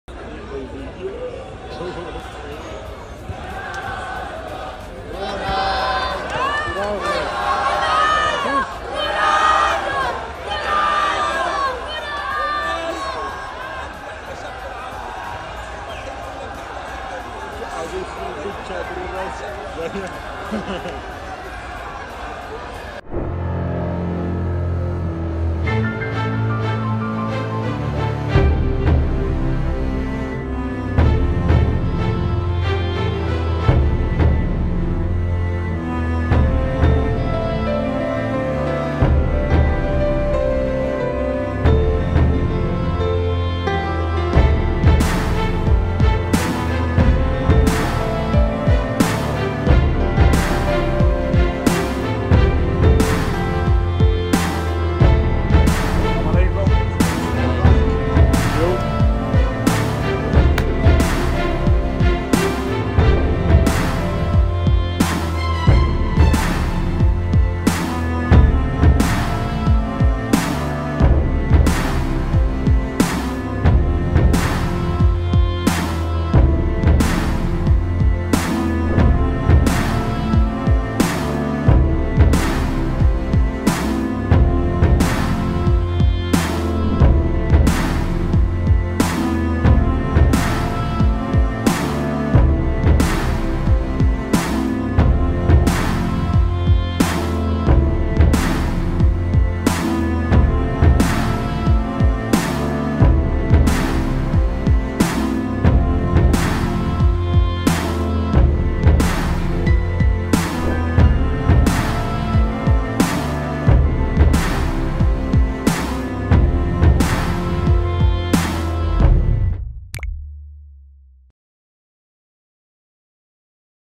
هتف باسمه والدموع تنهمر من عينيه.. كريستيانو رونالدو يبحث عن هذا الرجل المسن 💛
يبحث الأسطورة البرتغالية كريستيانو رونالدو قائد النصر عن مشجع مسن كان يهتف باسمه والدموع تنهمر من عينيه عند رؤيته لأول مرة على أرض الملعب.